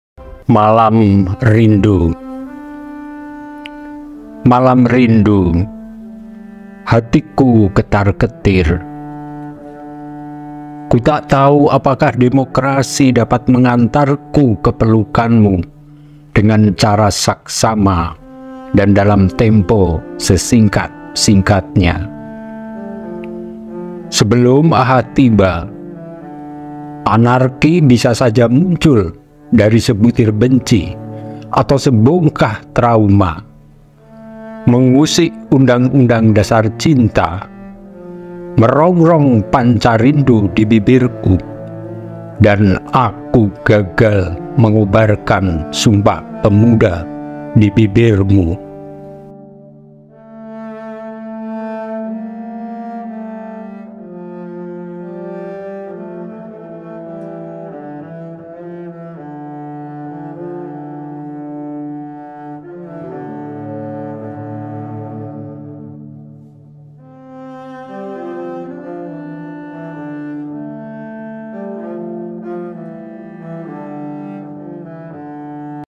PUISI : MALAM RINDU karya JOKO PINURBO | DIBACAKAN OLEH JOKO PINURBO
MALAM RINDU
Karya JOKO PINURBO

Malam Minggu. Hatiku ketar-ketir.
Ku tak tahu apakah demokrasi dapat mengantarku
ke pelukanmu dengan cara seksama
dan dalam tempo yang sesingkat-singkatnya.

Sebelum Ahad tiba, anarki bisa saja muncul
dari sebutir dengki atau sebongkah trauma,
mengusik undang-undang dasar cinta, merongrong
pencarindu di bibirku, dan aku gagal
mengobarkan Sumpah pemuda di bibirmu.